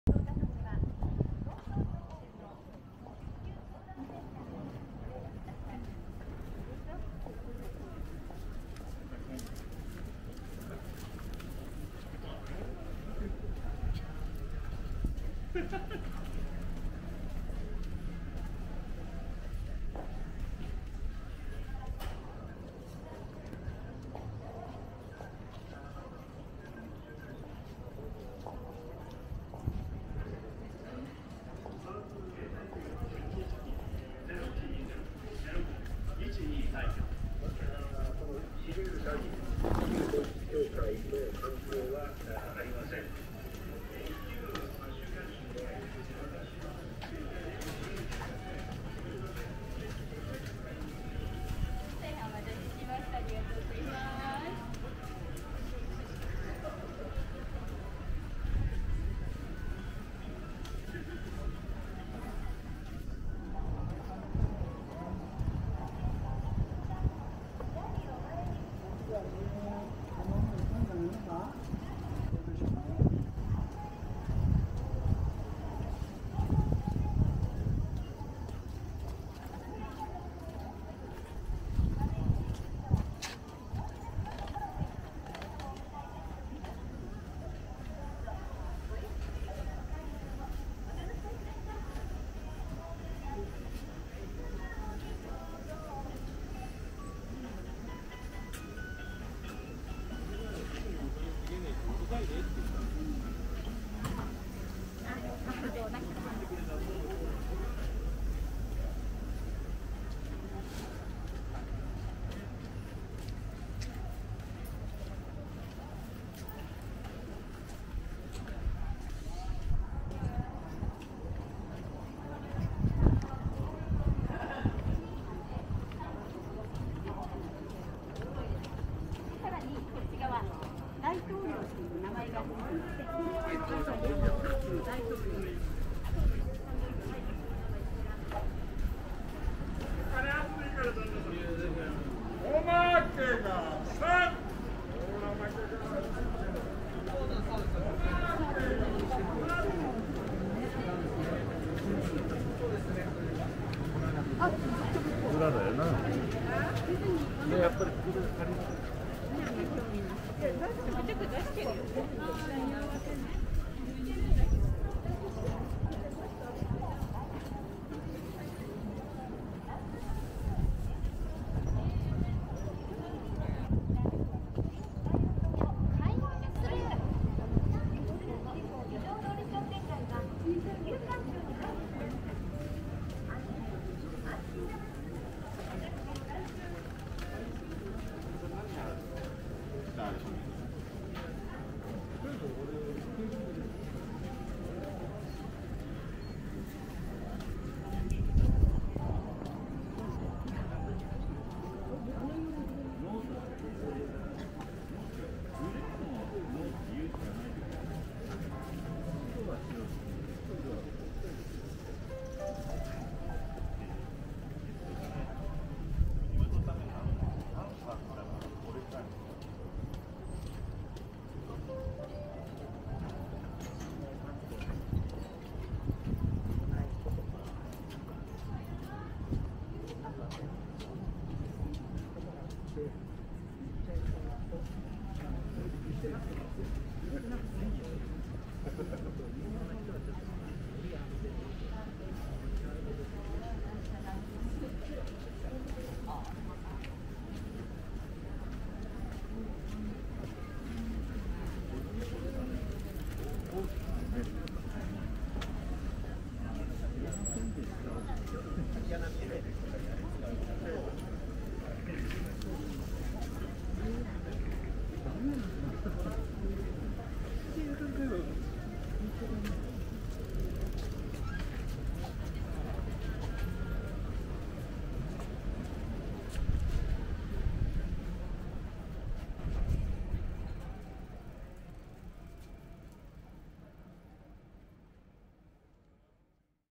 巣鴨【4K】巣鴨地蔵通り商店街を歩く Walking along Jizo-dori Shopping Street in Sugamo
I strolled along the Jizo-dori shopping street in Sugamo, known as "Grandma's Harajuku".
「おばあちゃんの原宿」として知られる巣鴨の地蔵通り商店街を散策しました。

◇Shooting date: August 24, 2022
◇Equipment:　DJI pocket2